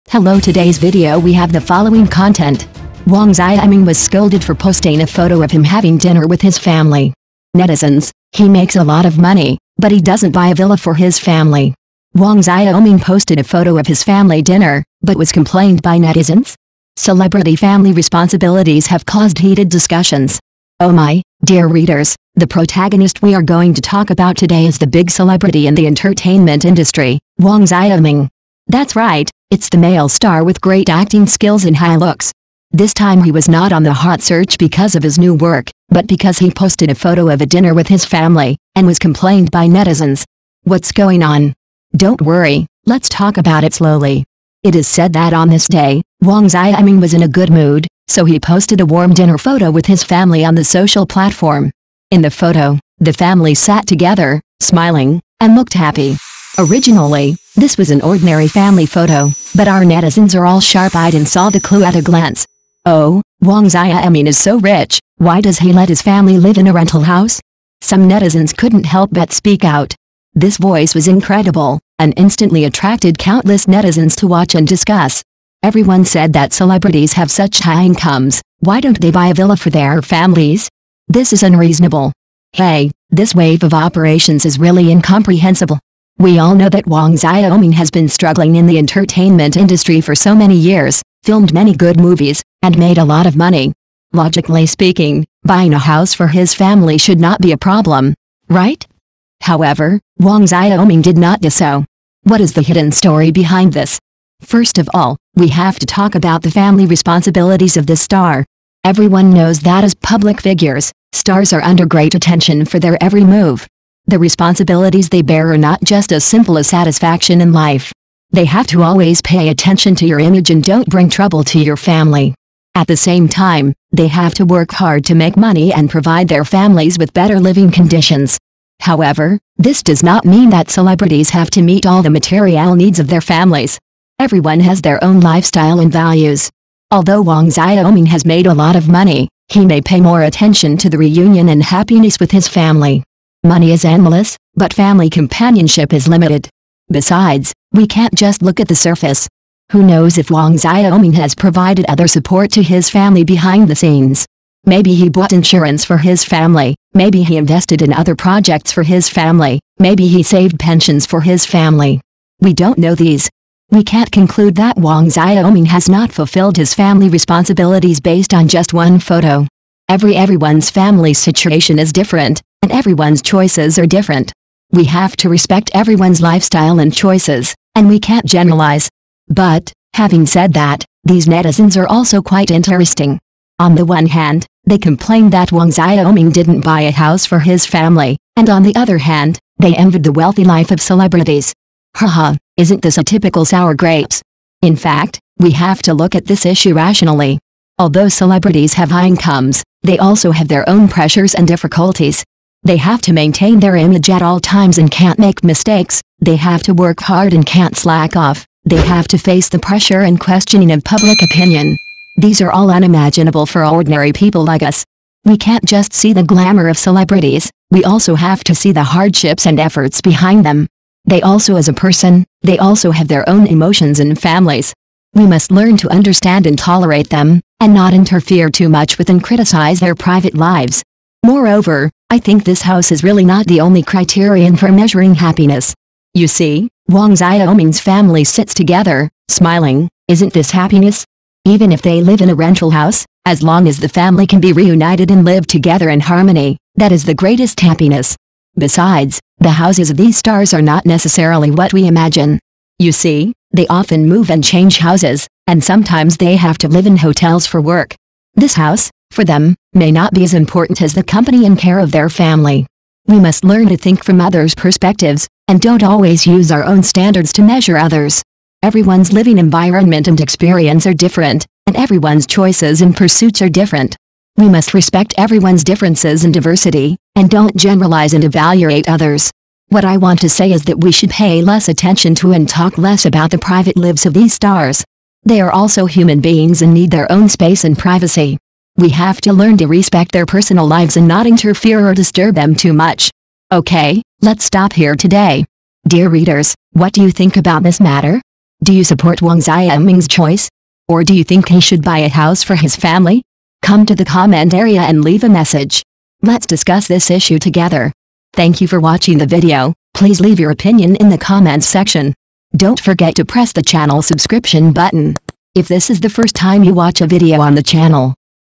Is 黄晓明's Decision to Not Buy a House for His Family a HUGE Mistake?
Huang Xiaoming was criticized for posting a photo of him eating with his family! Netizens: He makes a lot of money but doesn't buy a villa for his family